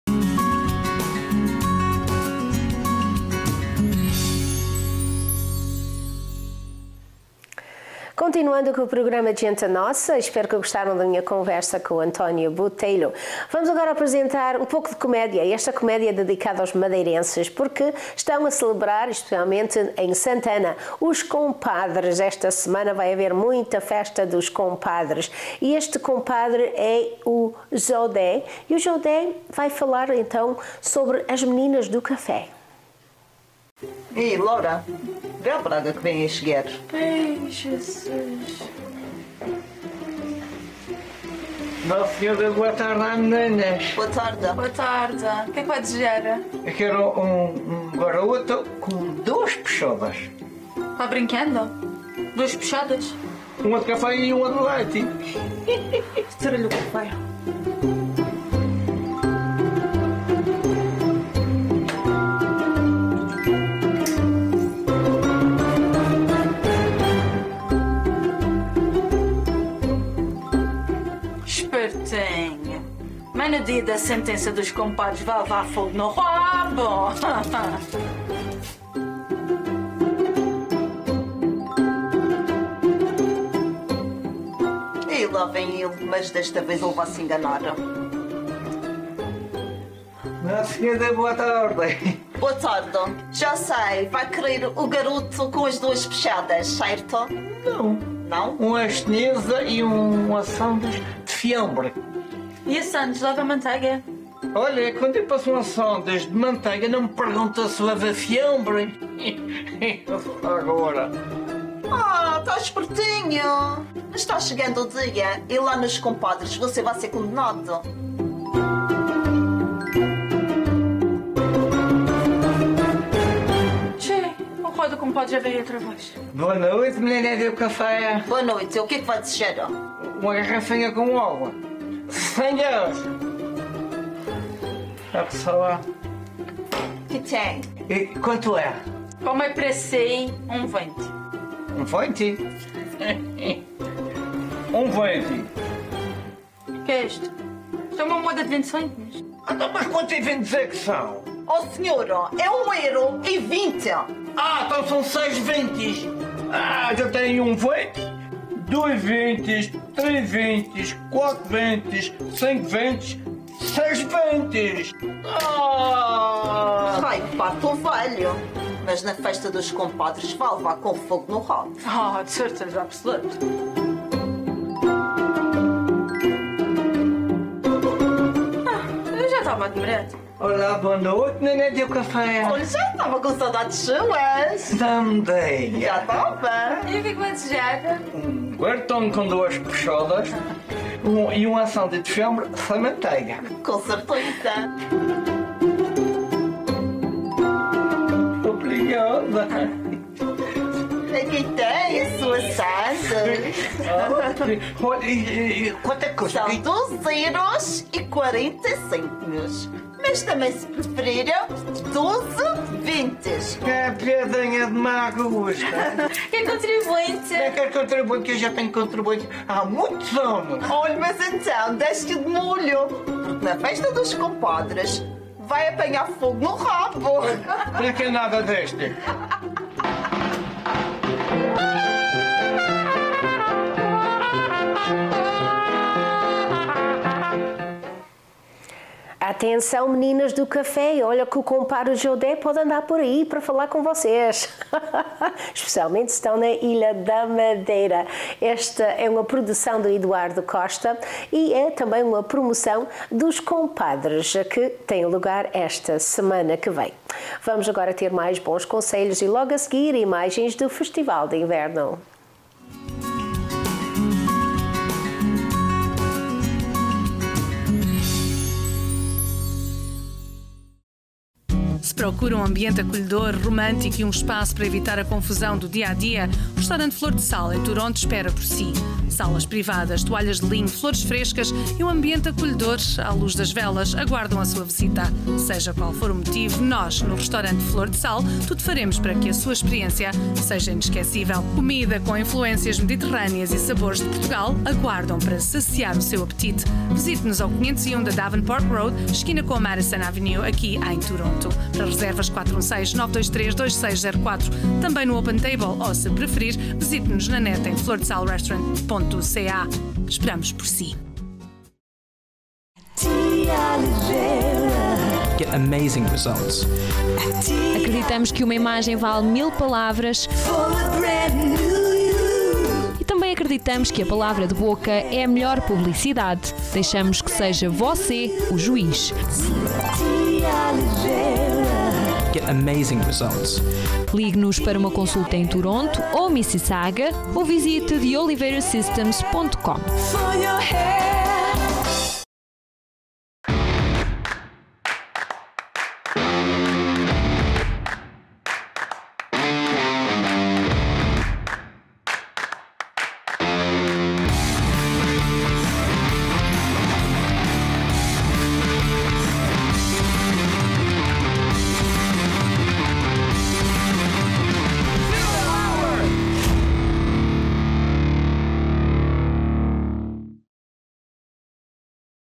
Festa dos Compadres com Jode' e as Meninas do Cafe' - Feb.18_23 - #3
Festa dos Compadres em Santana com o Compadre do Jode' e as Meminas do Cafe'.